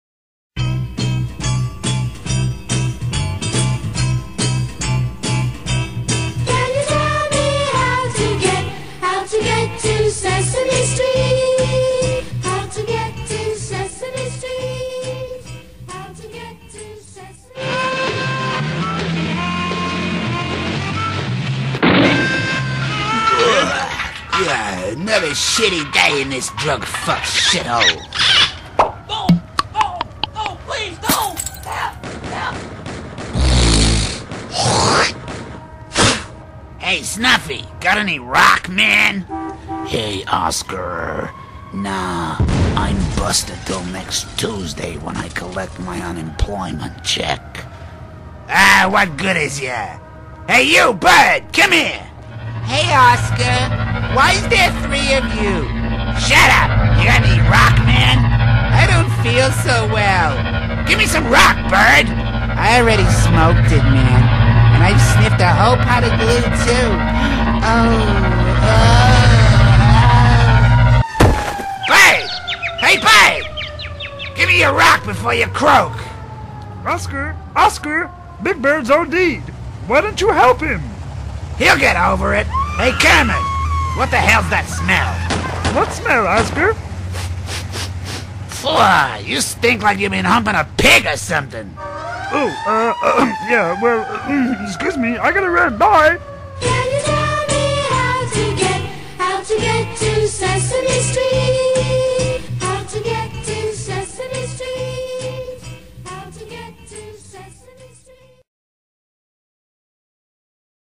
Sesame St. ' Hood--Kermit stinks.wmv
ShipOFools Radio Comedy Pirates.Sesame St parody. Sesame Street like you've never seen before